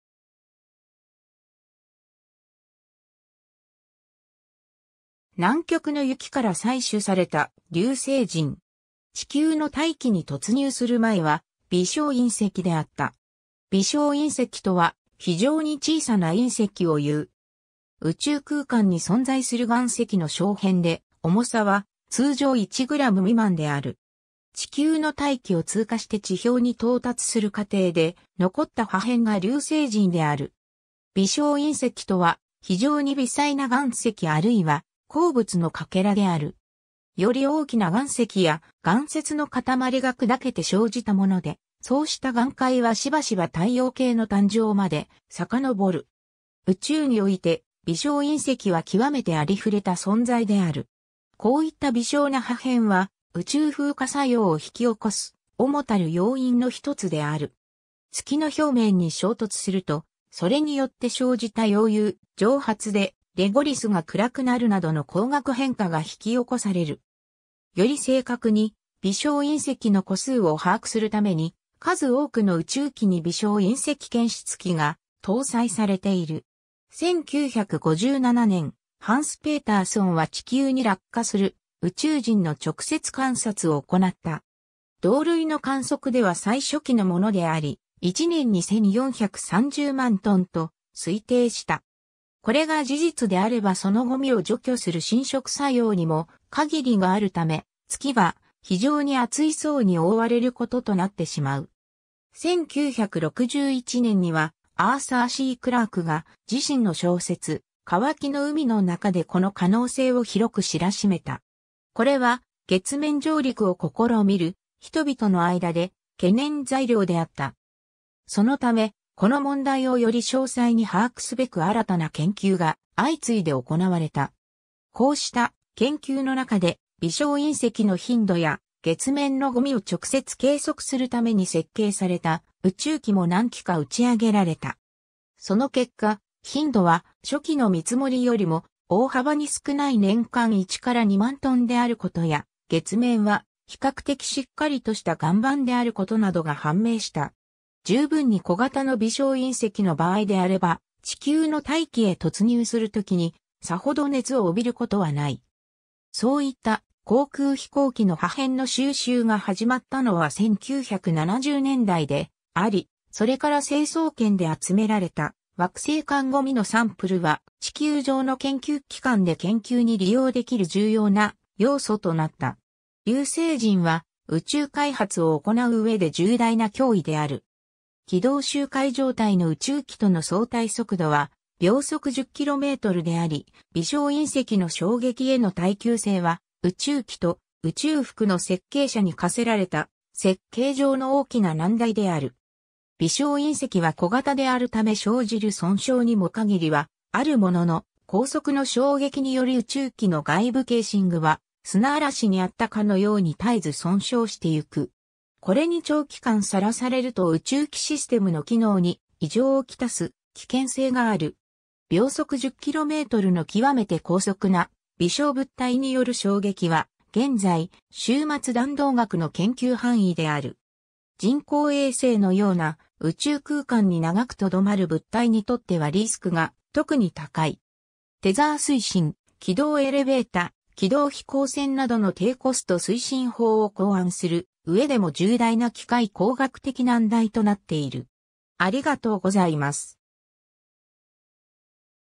微小隕石
南極の雪から採取された流星塵。
地球の大気に突入する前は微小隕石であった。
 微小隕石（びしょういんせき）とは非常に小さな隕石をいう。
宇宙空間に存在する岩石の小片で、重さは通常1グラム未満である。
地球の大気を通過して地表に到達する過程で残った破片が流星塵である。
 微小隕石とは非常に微細な岩石あるいは鉱物の欠片である。
より大きな岩石や岩屑の塊が砕けて生じたもので、そうした岩塊はしばしば太陽系の誕生までさかのぼる。
宇宙において微小隕石は極めてありふれた存在である。
こういった微小な破片は宇宙風化作用を引き起こす主たる要因の1つである。
月(あるいは水星や小惑星などといった無大気天体)の表面に衝突すると、それによって生じた溶融・蒸発でレゴリスが暗くなるなどの光学変化が引き起こされる。
より正確に微小隕石の個数を把握するために、数多くの宇宙機（ルナ・オービター1号、ルナ3号、マルス1号、パイオニア5号など）に微小隕石検出器が搭載されている。
 1957年、ハンス・ペーターソンは地球に落下する宇宙塵の直接観察を行った。
同類の観測では最初期のものであり、1年に1430万トンと推定した。
これが事実であればその塵を除去する浸食作用にも限りがあるため、月は非常に厚い層に覆われることとなってしまう。
1961年にはアーサー・C・クラークが自身の小説『渇きの海』の中でこの可能性を広く知らしめた。
これは月面上陸を試みる人々の間で懸念材料であった。
そのため、この問題をより詳細に把握すべく新たな研究が相次いで行われた。
こうした研究の中で、微小隕石の頻度（サーベイヤー計画）や月面の塵（人工衛星ペガサス計画）を直接計測するために設計された宇宙機も何機か打ち上げられた。
その結果、頻度は初期の見積もりよりも大幅に少ない年間1～2万トンであることや、月面は比較的しっかりとした岩盤であることなどが判明した。
 十分に小型の微小隕石の場合であれば地球の大気へ突入する時にさほど熱を帯びることはない。
そういった高空飛行機の破片の収集が始まったのは1970年代であり、それから成層圏で集められた惑星間塵（起源が地球外であると確認されるまでブラウンリー粒子と呼ばれた）のサンプルは地球上の研究機関で研究に利用できる重要な要素となった。
 流星塵は宇宙開発を行う上で重大な脅威である。
軌道周回状態の宇宙機との相対速度は秒速10キロメートル（すなわち時速2万2500キロメートル）であり、微小隕石の衝撃への耐久性は宇宙機と宇宙服の設計者に課せられた設計上の大きな難題である（宇宙環境保護層を参照）。
微小隕石は小型であるため生じる損傷にも限りはあるものの、高速の衝撃により宇宙機の外部ケーシングは砂嵐に遭ったかのように絶えず損傷してゆく。
これに長期間さらされると宇宙機システムの機能に異常を来す危険性がある。
 秒速10キロメートルの極めて高速な微小物体による衝撃は現在、終末弾道学の研究範囲である（こうした速度まで物体を加速させるのは困難である。現在の技術にはレールガンや成形炸薬がある）。
人工衛星のような、宇宙空間に長くとどまる物体にとってはリスクが特に高い。
テザー推進、軌道エレベータ、軌道飛行船などの低コスト推進法を考案する上でも重大な機械工学的難題となっている。